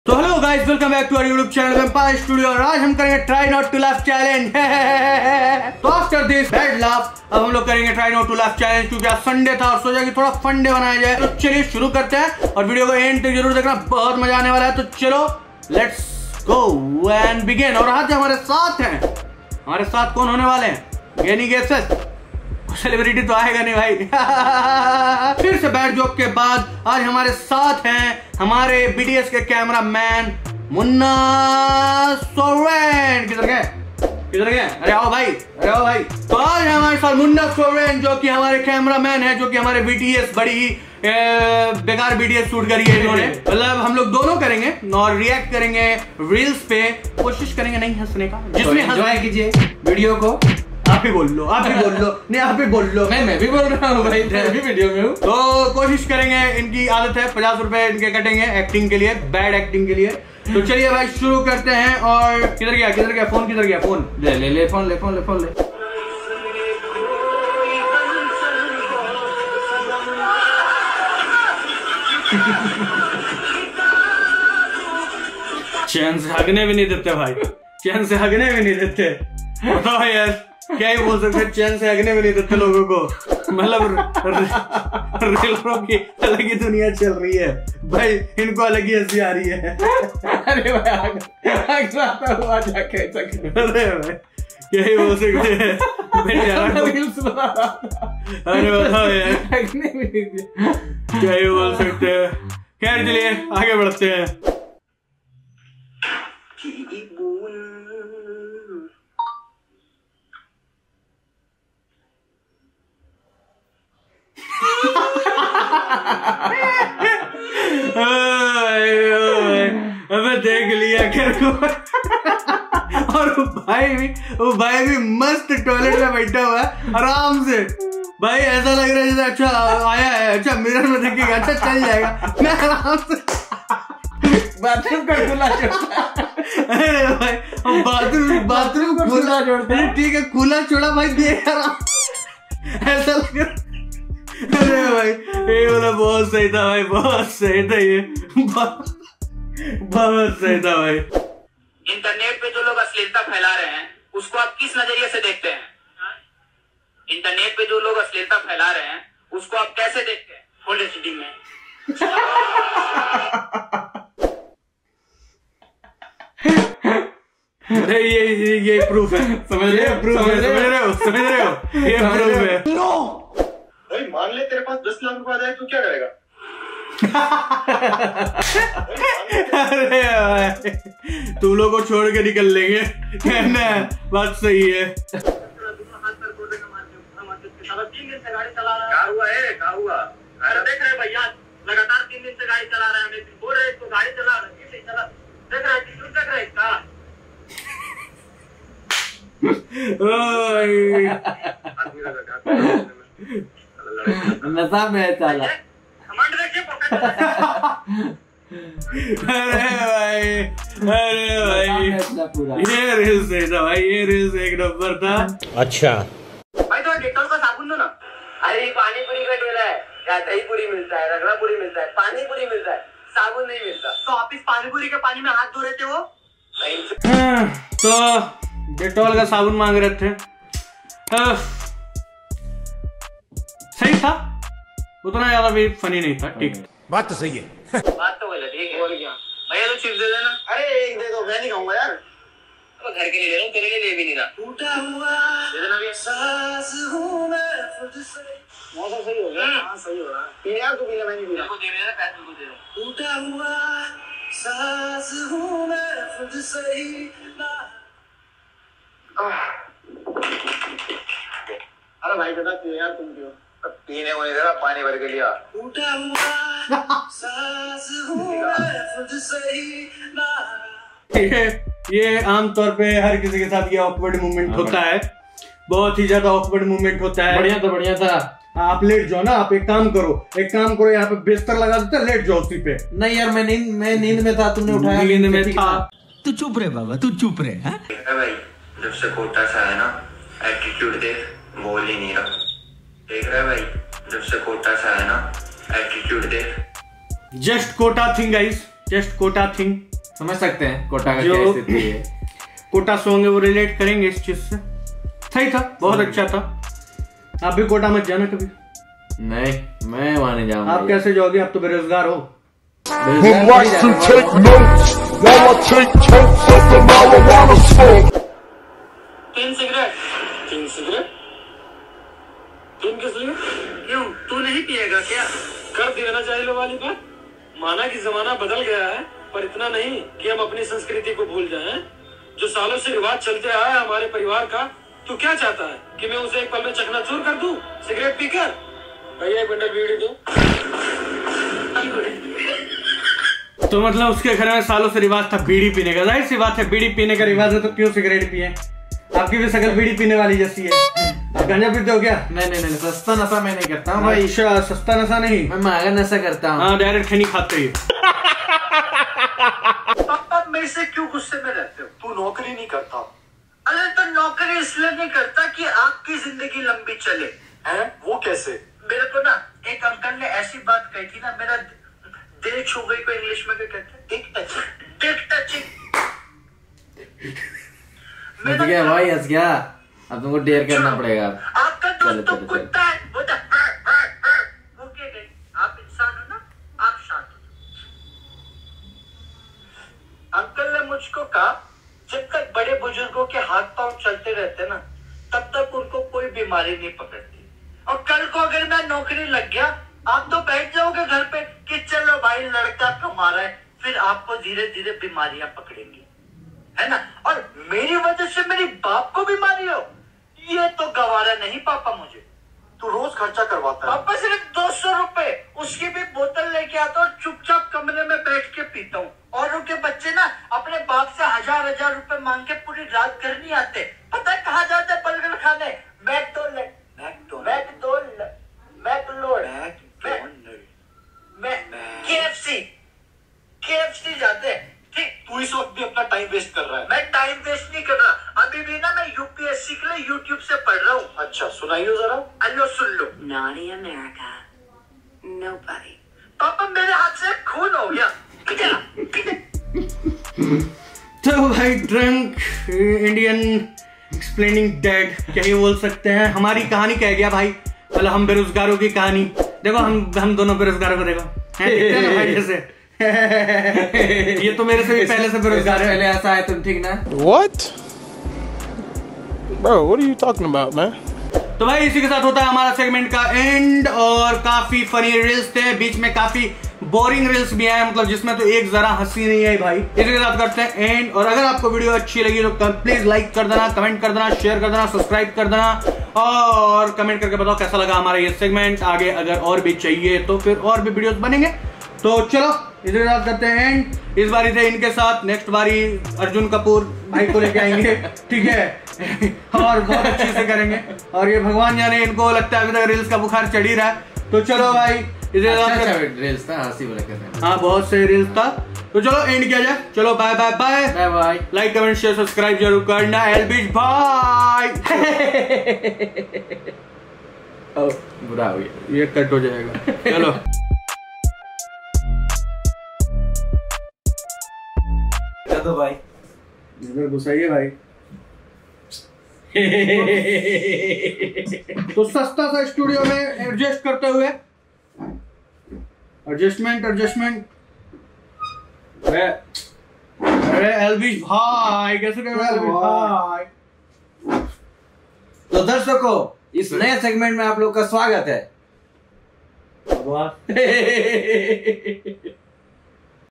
Hugne Do Bhai Chain Se 🥲 | Try Not To Laugh Challenge | Funniest Challenge Ever 😂
In This Video We Are Doing A Try Not To Laugh Challenge On Some Funny Memes From The Internet